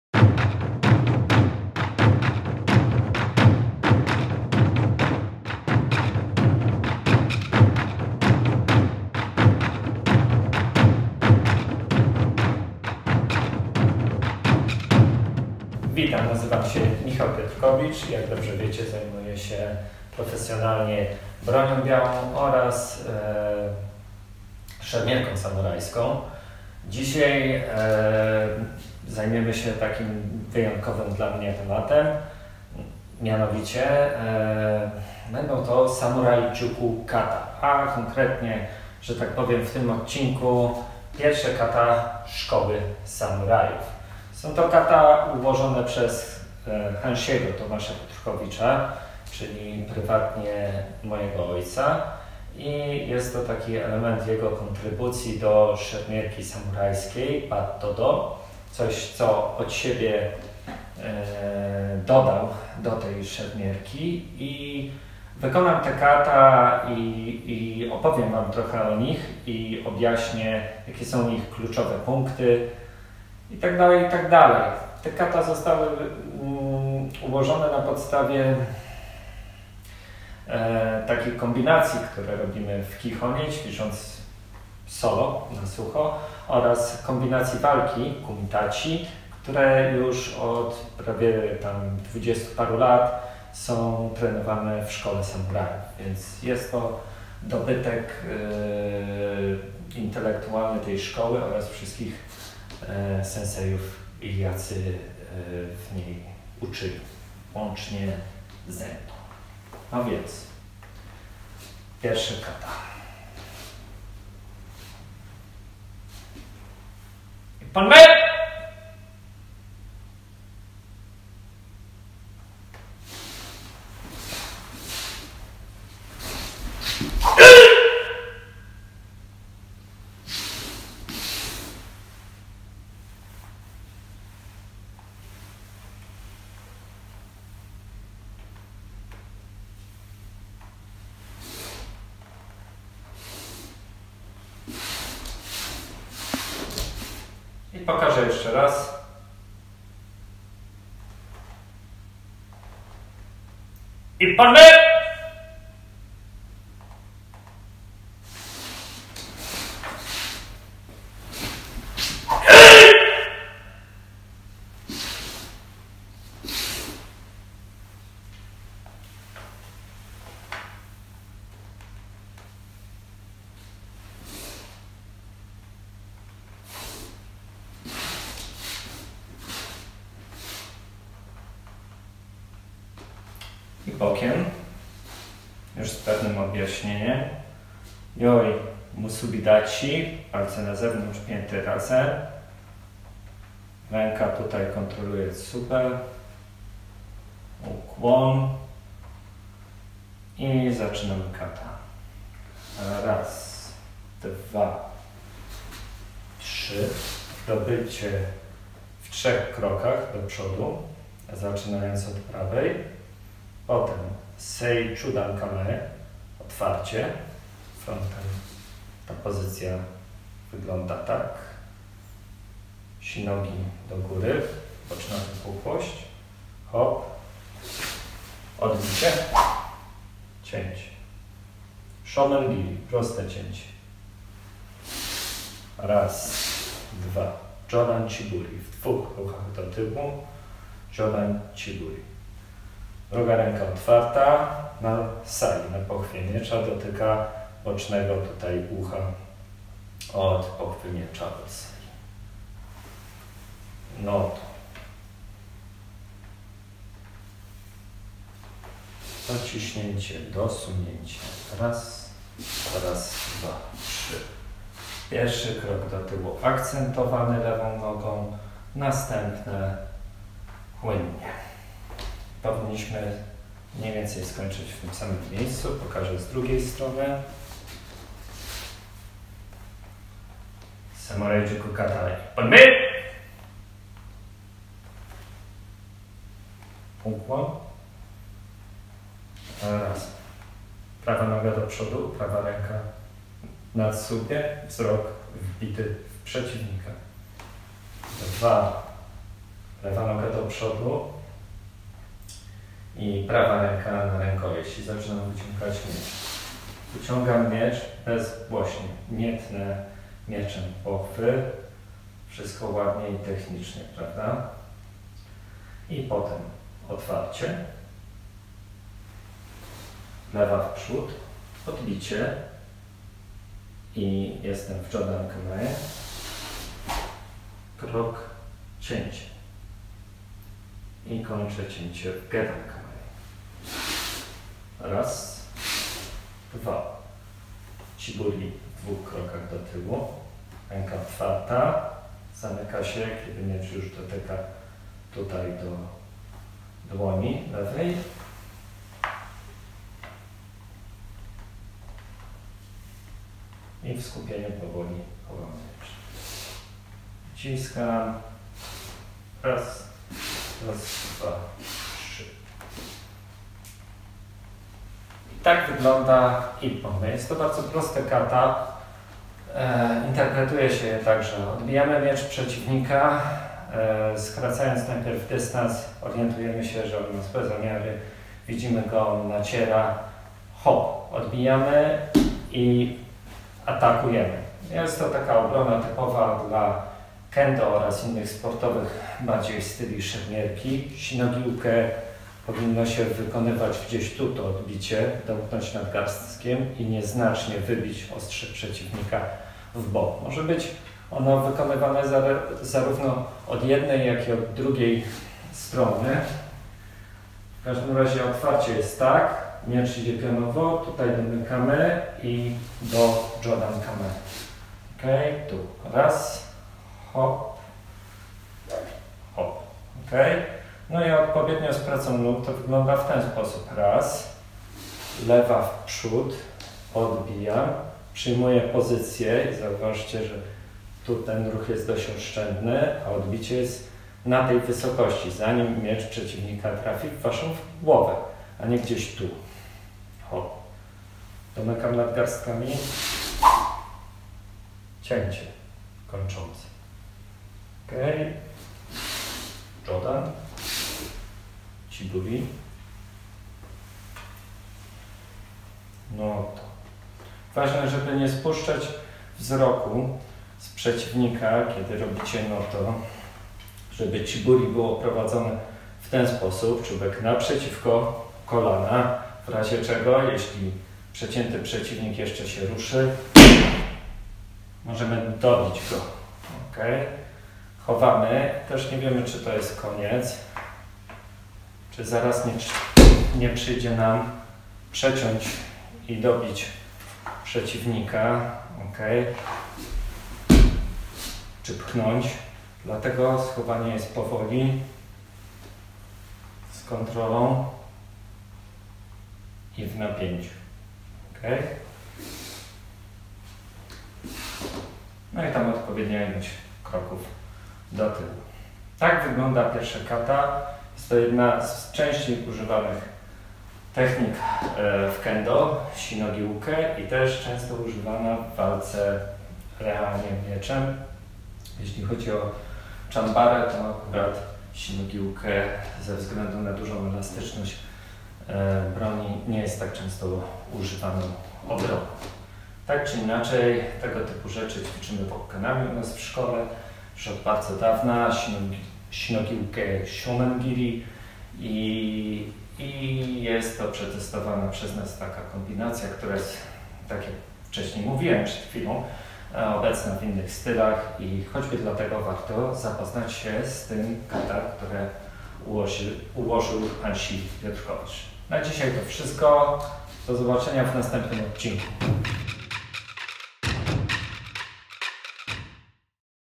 Pierwsza forma Szkoły Samurajów
Oto pierwsza z form mojej macierzystej szkoły szermierki (w sensie klubu). Przedstawiam wam Samurai Jukku Kata: Ippon me. Jak można spodziewać się po pierwszej formie jest bardzo prosta.
Polecam z całego szermierczego serca!